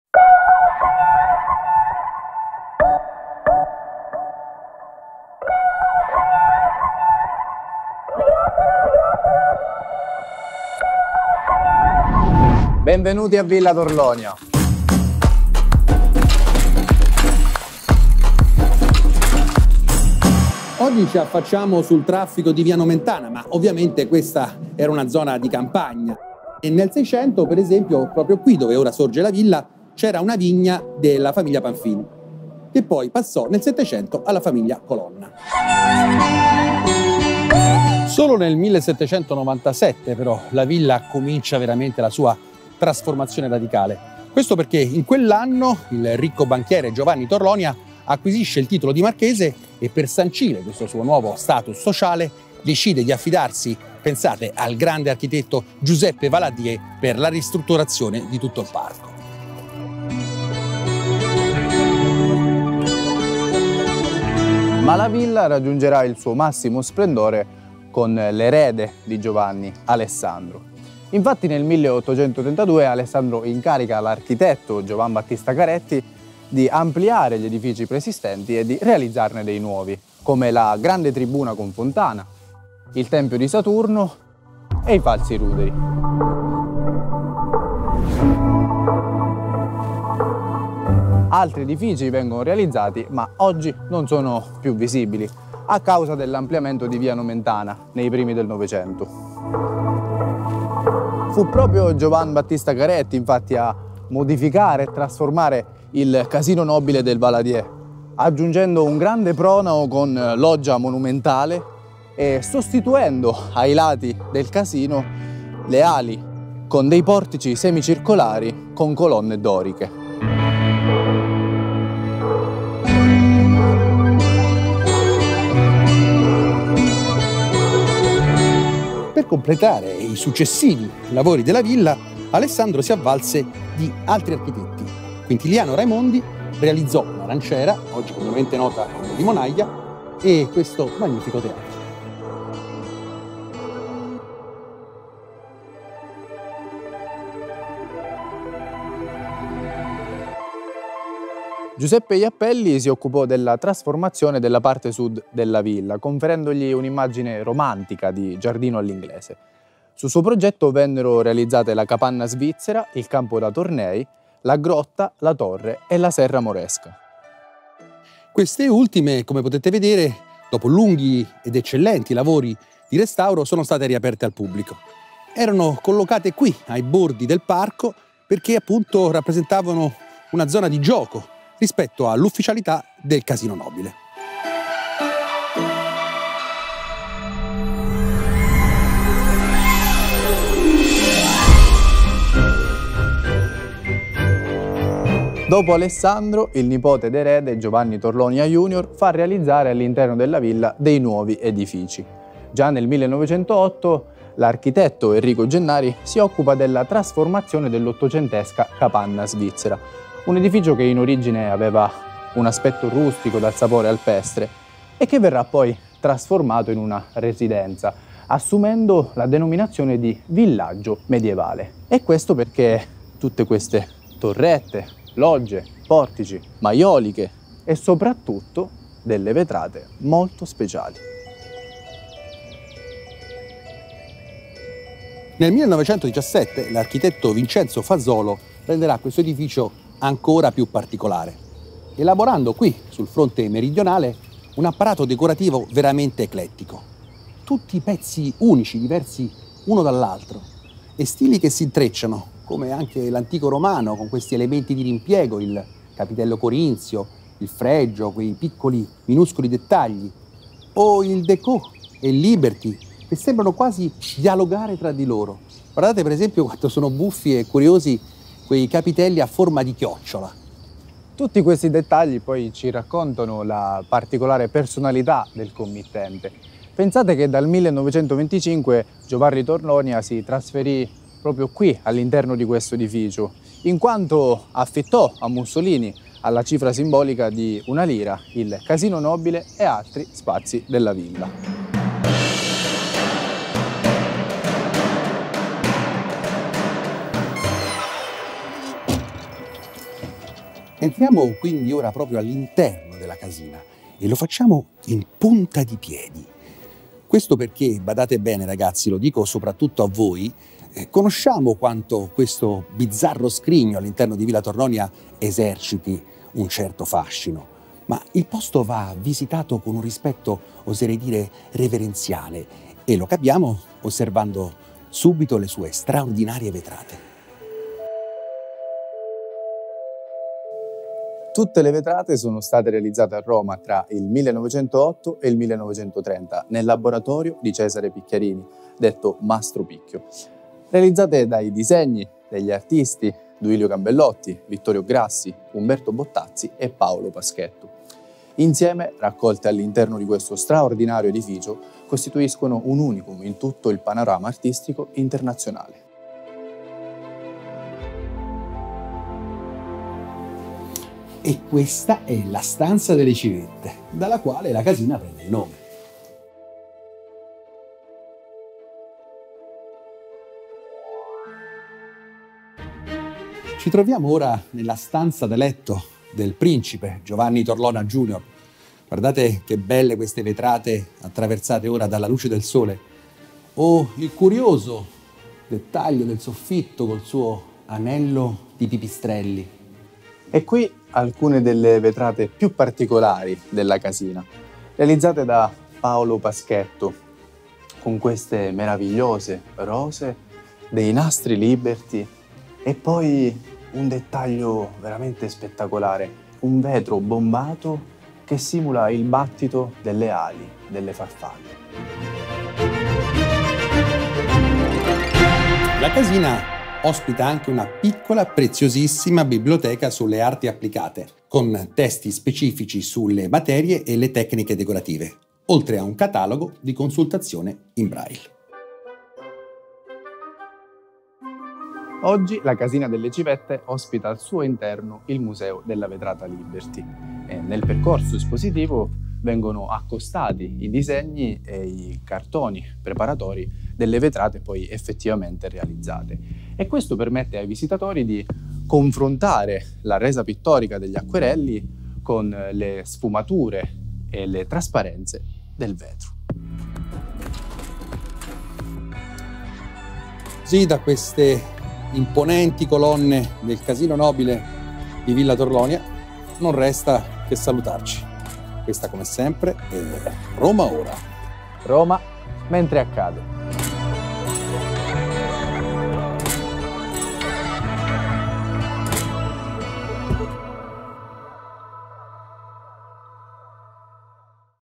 RomaOra racconta VILLA TORLONIA e la CASINA delle CIVETTE
Prosegue il nostro viaggio alla scoperta dei tesori del II Municipio di Roma Capitale.

Tra i parchi e il verde che il II Municipio ospita nel suo territorio, si trova l'affascinante Villa Torlonia. All'inizio di Via Nomentana, poco distante da Porta Pia, un luogo che non è semplicemente un'area verde del Quartiere Nomentano, ma rappresenta soprattutto un importante polo culturale. Musei, aree espositive, un teatro, archivi e biblioteche. Luoghi unici da non perdere a Roma.

Questo video fa parte di un progetto di valorizzazione del territorio del II Municipio di Roma Capitale. Si ringraziano Francesca Di Bello presidente del II Municipio, Emanuele Gisci assessore e vicepresidente, l’assessora Paola Rossi, l’assessore Rino Fabiano, la consigliera Valentina Caracciolo e il consigliere Gianluca Bogino.

Riprese e Montaggio: PixelCake Visual Factory
Testi: Andrea Comisi e Marco Fiocchi